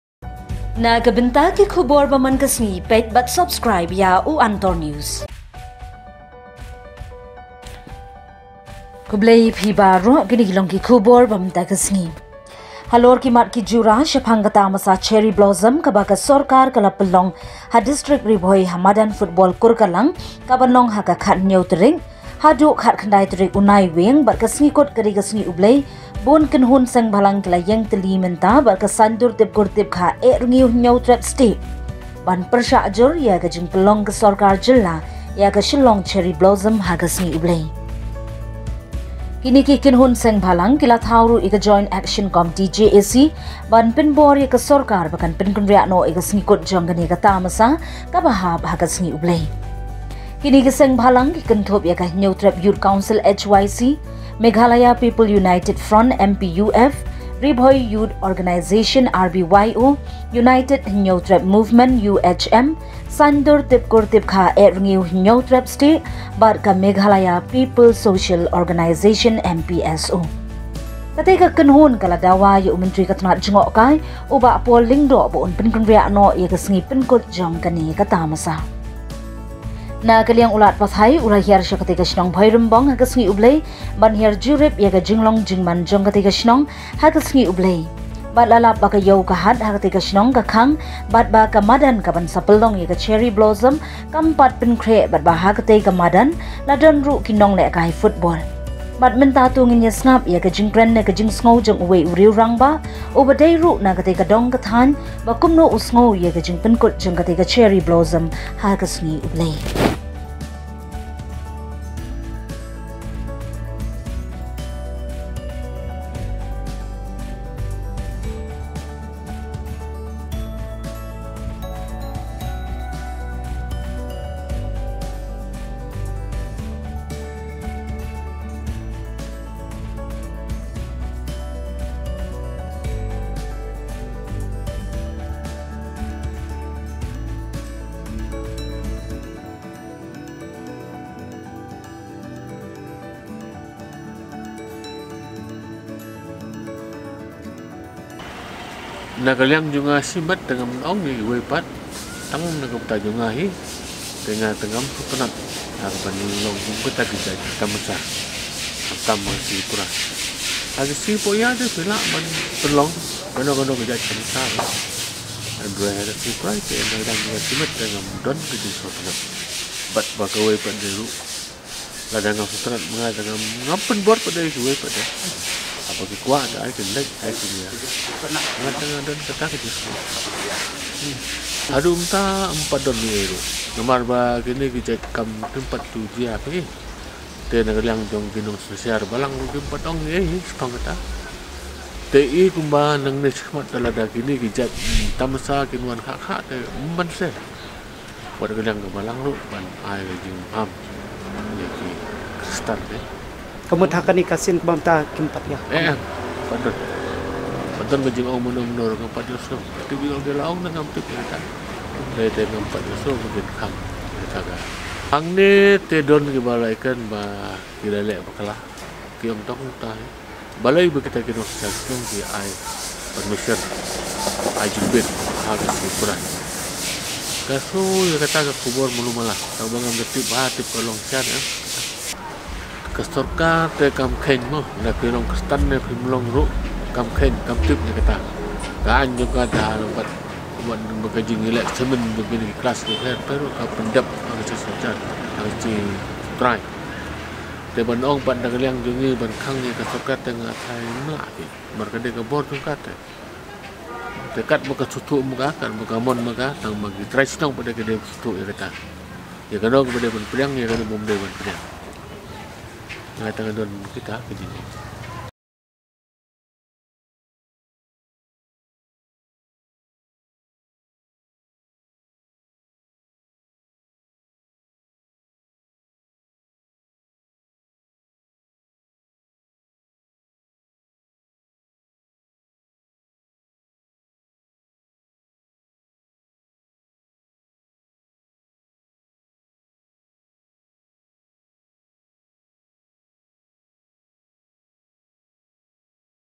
"Ka sorkar kam khein, phi long khristan ne ym long" pat ong ei ei ki Balang (Cherry Blossom Fest.)
ki khubor na ka jylla